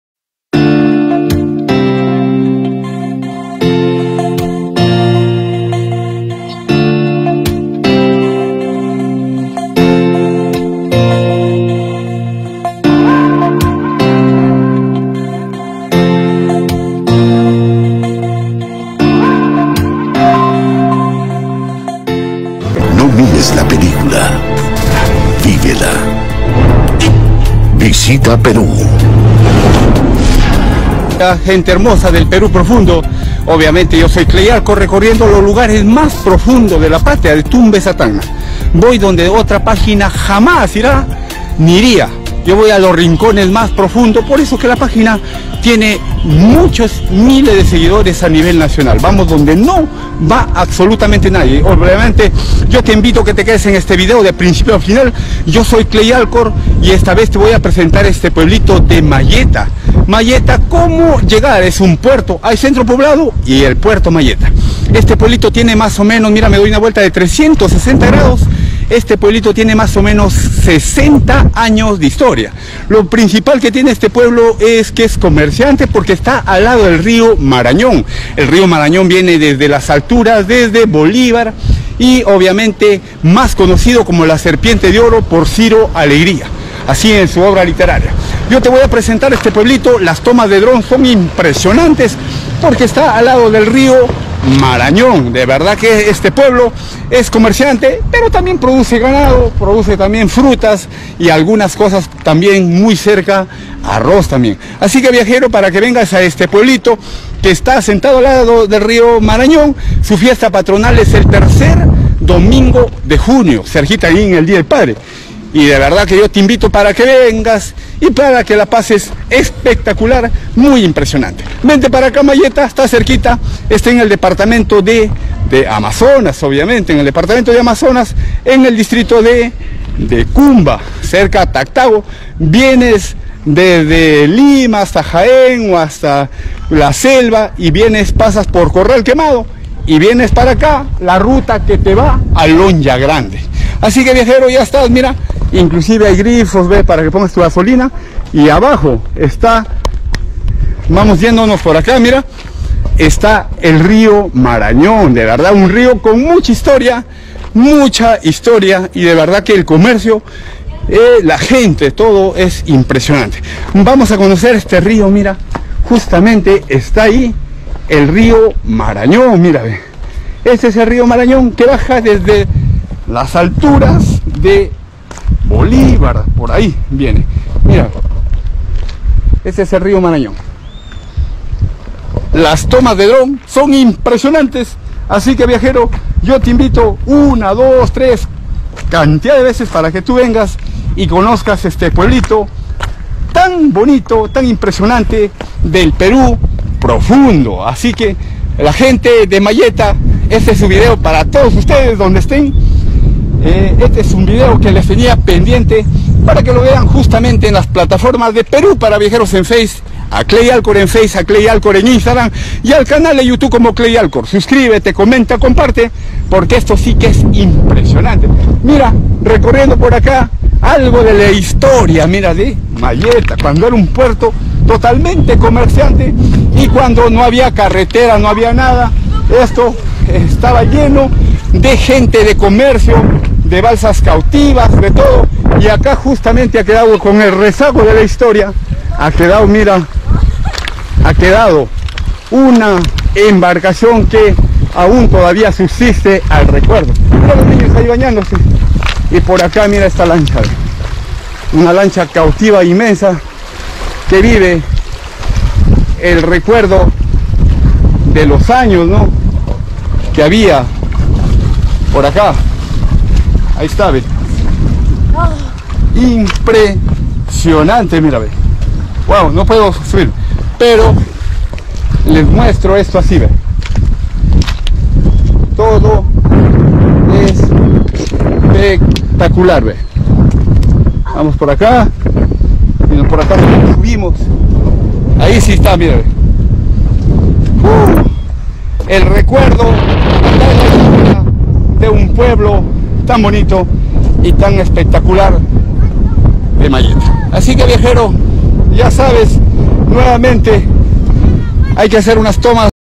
Puerto Malleta-- YAMÓN -UTCUBAMBA -Amazonas. ¿CÓMO LLEGAR PASO A PASO?
☀️ INTRODUCCIÓN N
Este lugar hermoso asentado al lado del río Marañón, se llama Puerto Mayeta, tiene en su fortaleza el comercio y la pesca ,este distrito pertenece al distrito de Cumba y a la provincia de Utcubamba ,este centro poblado su fiesta patronal es el tercer domingo de junio